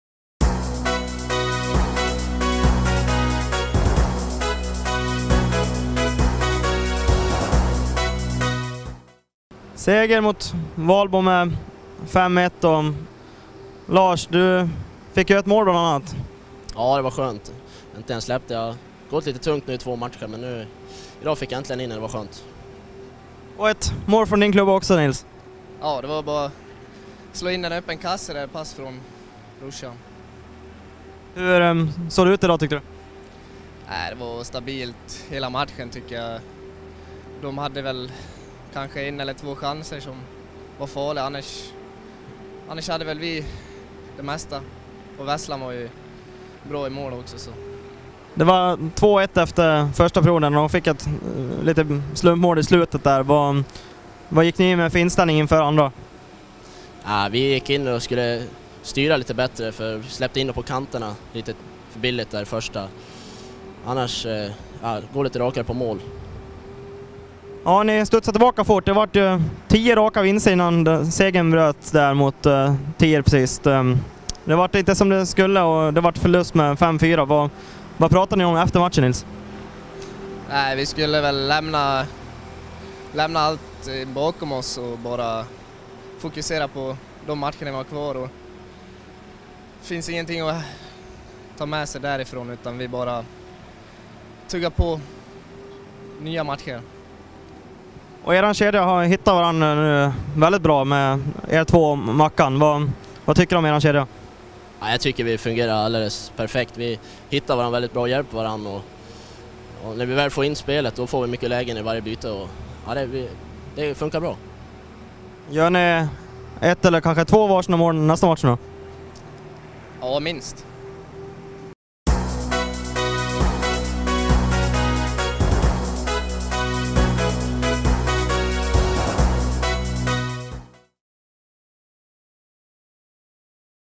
Canucks har tvillingarna Sedin, Borlänge tvillingarna Nils o Lars Eriksson
Lars och Nils Eriksson efter segermatchen  mot Valbo.
Intervjuerna med tvillingarna och Niklas Westling innehåller alla målen från matchen
BHF-Valbo Hockey